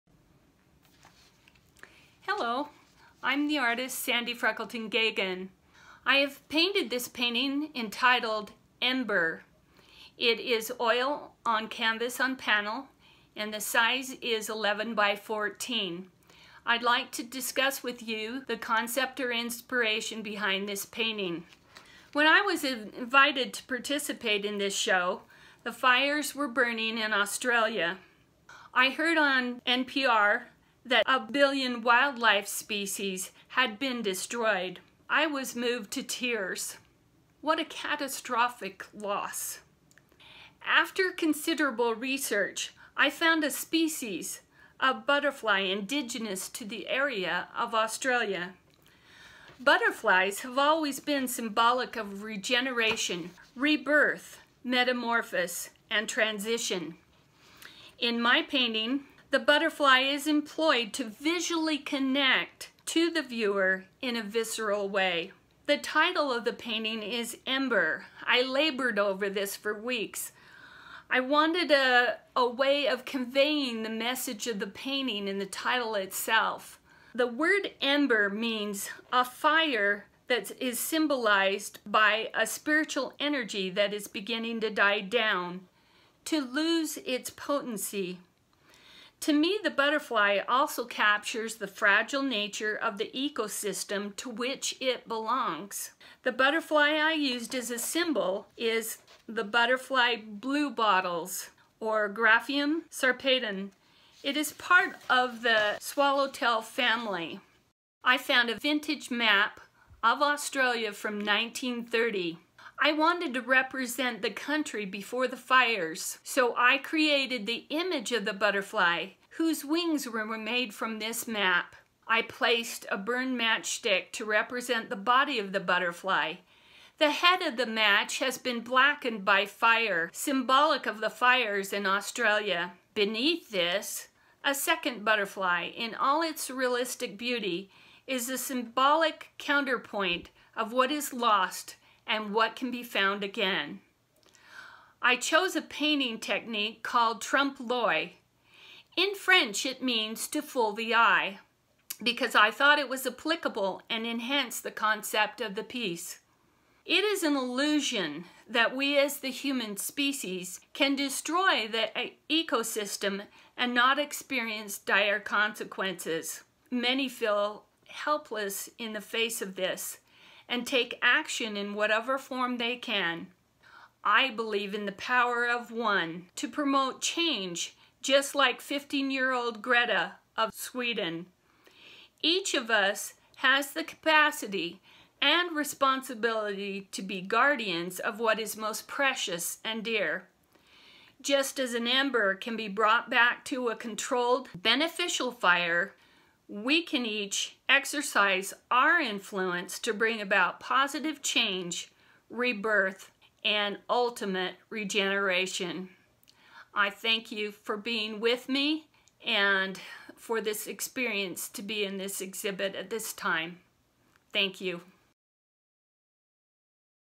Sandy Freckleton Gagon, on OUR GLOBAL CLIMATE CRISIS group show
Artist Sandy Freckleton Gagon walks us through the concept behind her latest work in our climate-themed group exhibition. Big thank you to Sandy for sending us a video from home!

This particular painting is already sold. However, please go to our website for more works, from this exhibit and other artists. We appreciate your support in local artists.